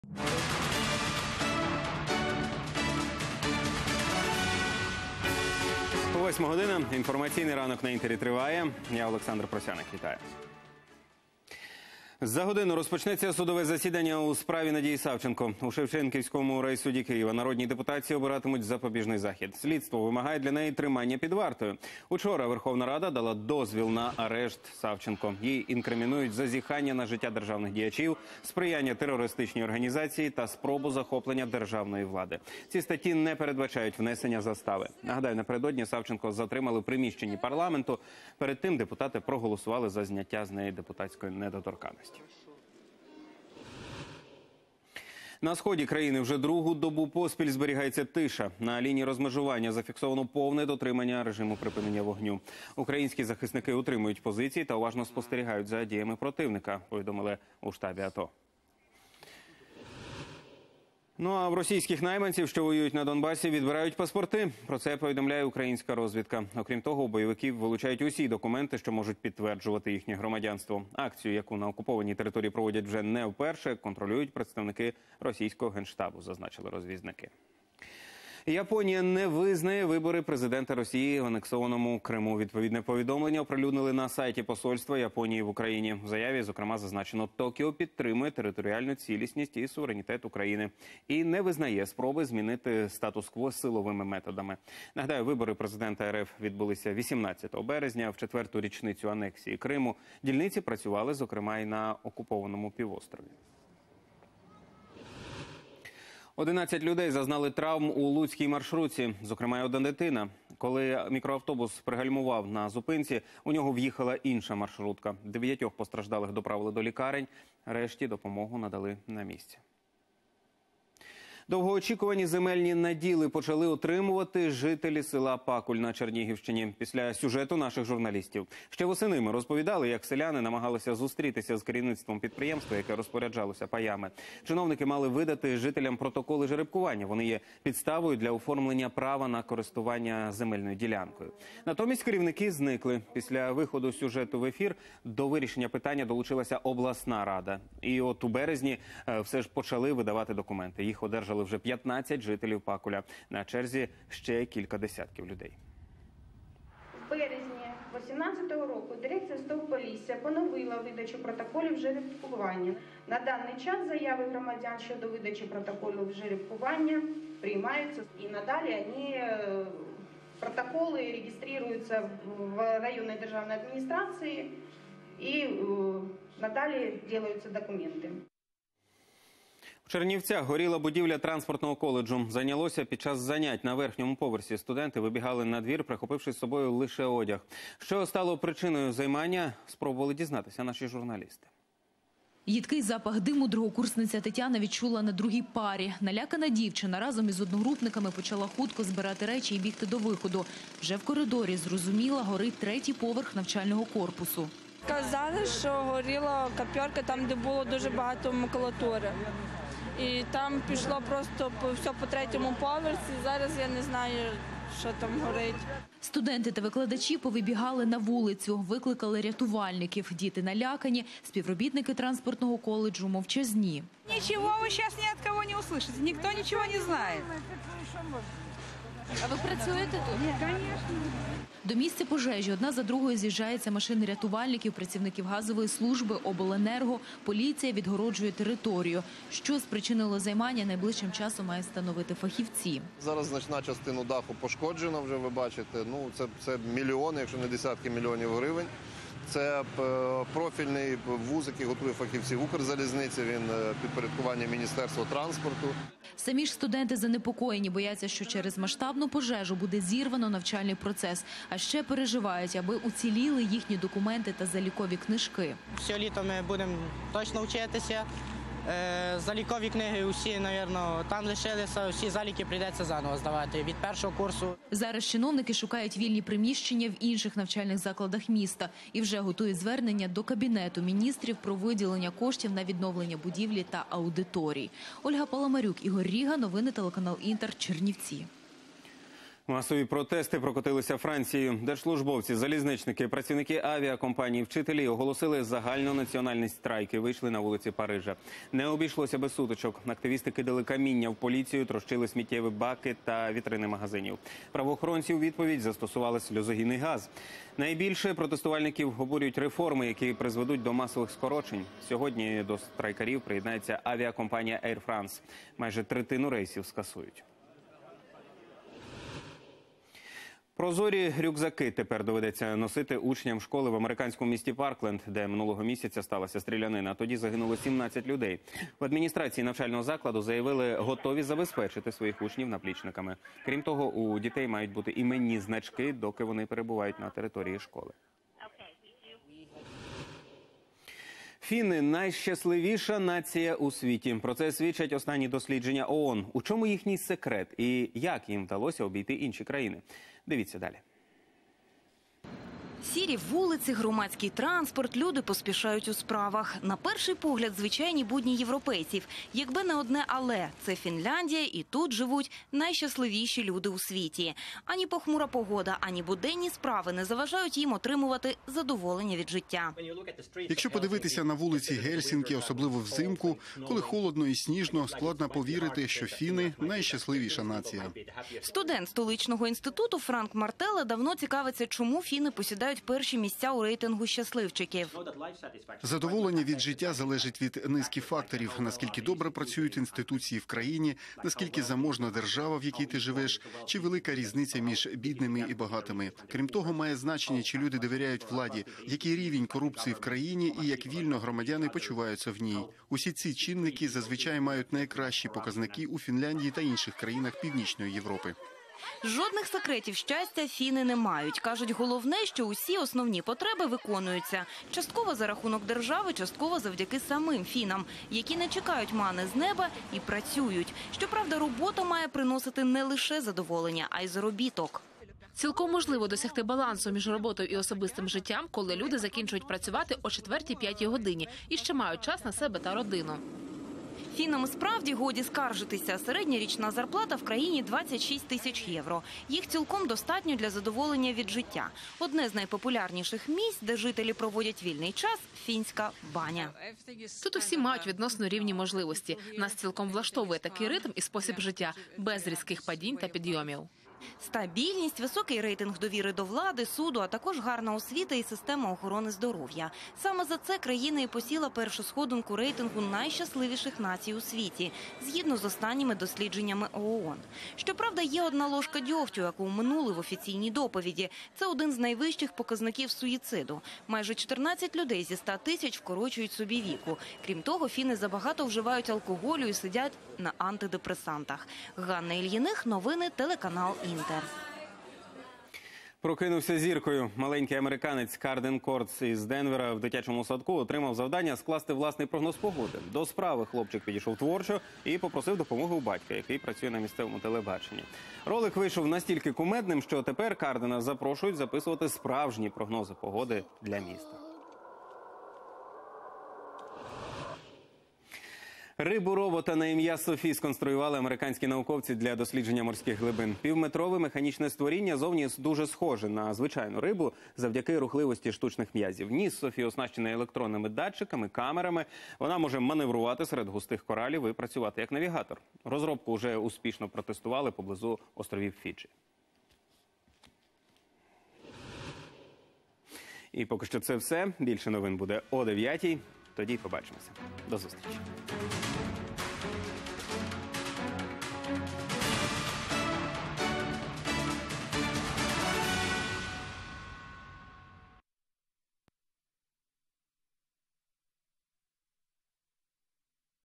Новости 8:00, выпуск за 23 марта 2018 года
Головні події на цю годину:
У суді обиратимуть запобіжний захід Надії Савченко; Бойовикам на Донбасі наказали здати всі російські документи - розвідка; Японія не визнає вибори президента Росії в анексованому Криму - МЗС; У Луцьку дві маршрутки не поділили зупинку; На Чернігівщині поновили видачу селянам земельних паїв; У Чернівцях горіла будівля транспортного коледжу; Францію сколихнули масові акції протесту; Фінляндію визнали "найщасливішою" країною світу; Прокинувся знаменитим: як маленький американець став зіркою місцевого телебачення (відео).